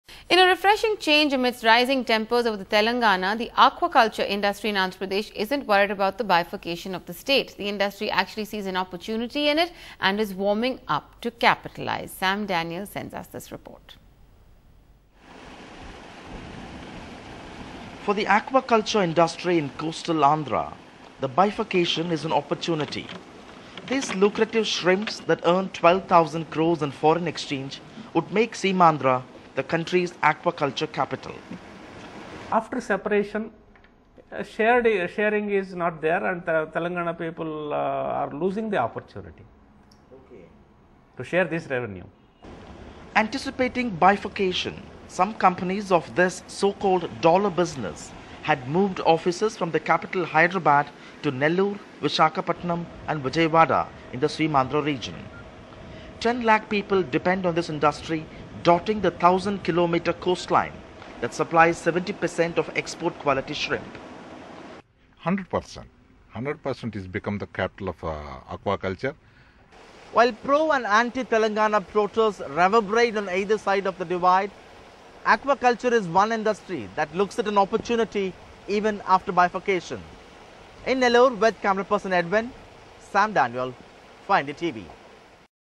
Why aqua industry is happy over division of Andhra Pradesh
In a refreshing change amidst rising tempers over Telengana, the aqua culture industry in Andhra Pradesh isn't worried about the bifurcation of the state. The industry actually sees an opportunity and is warming up to capitalise on it.